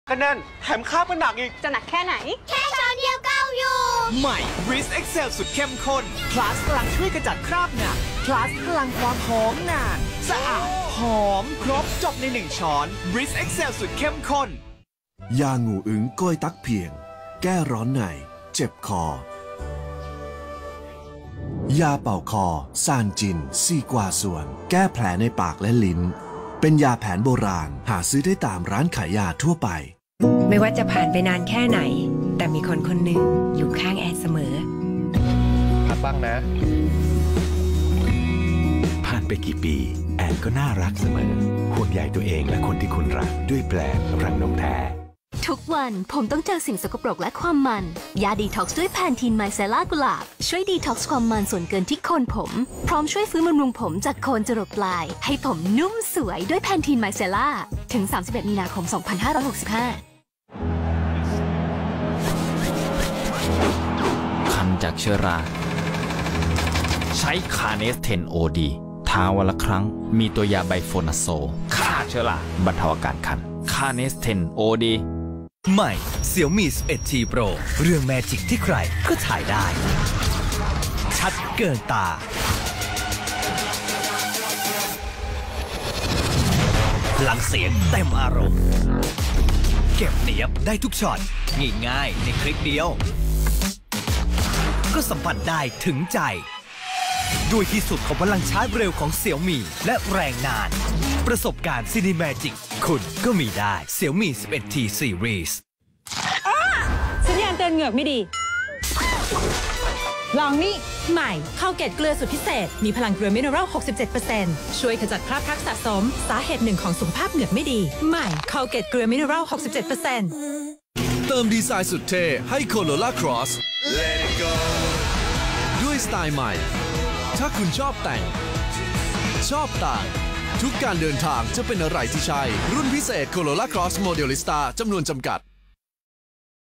โฆษณาไทย 2564 - 5 HD | คั่นรายการ ข่าวเย็นไทยรัฐ | ช่อง ไทยรัฐทีวี 32
โฆษณาคั่นรายการ ข่าวเย็นไทยรัฐ
ทางช่อง ไทยรัฐทีวี 32
20 ตุลาคม พ.ศ.2564

ป.ล.ห้ามกดไม่ชอบ ไม่ให้คนเห็น ให้ปิด dislike